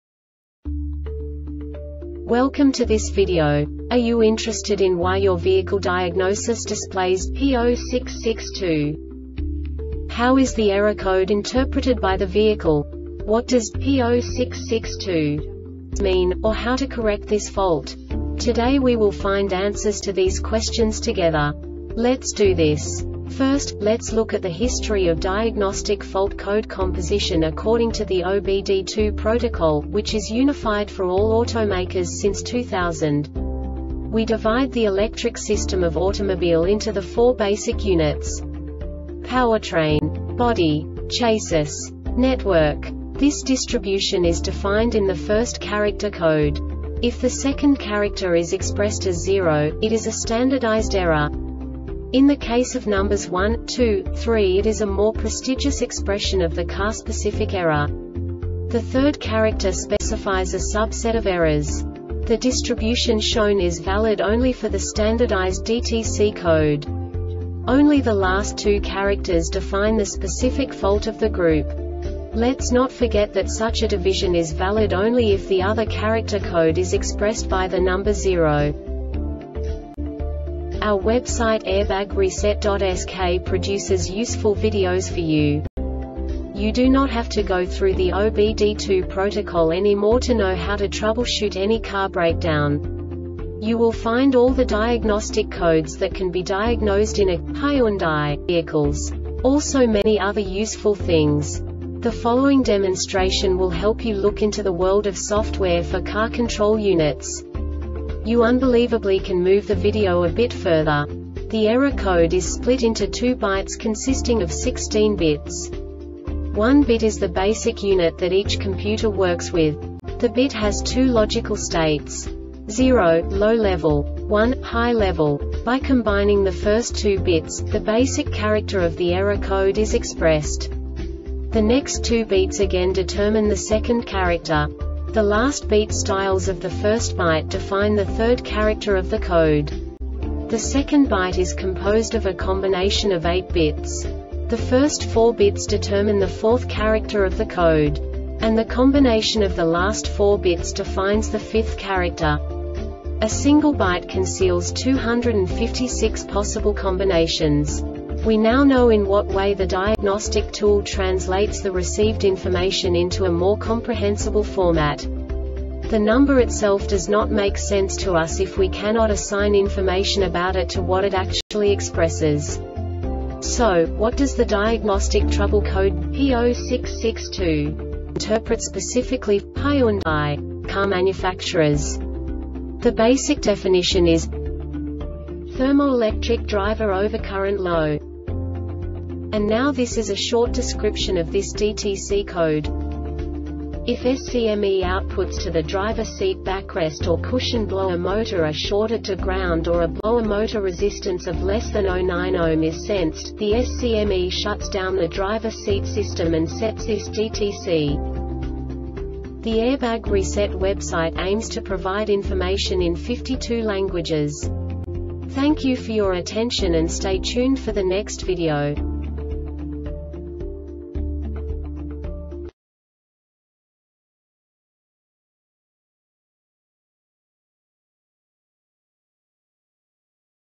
DTC Hyundai P0662 Short Explanation
Contents:
0:21 Basic DTC analysis according to OBD2 protocol standard.
1:48 Insight into programming
2:58 A diagnostically understandable description of the Diagnostic Trouble Code
3:05 Basic error definition

Make:
Hyundai
Code:
P0662
Definition:
Thermoelectric Driver Overcurrent Low
Cause:
If SCME outputs to the driver seat backrest or cushion blower motor are shorted to ground or a blower motor resistance of less than 0.9 ohm is sensed,the SCME shuts down the driver seat system and sets this DTC .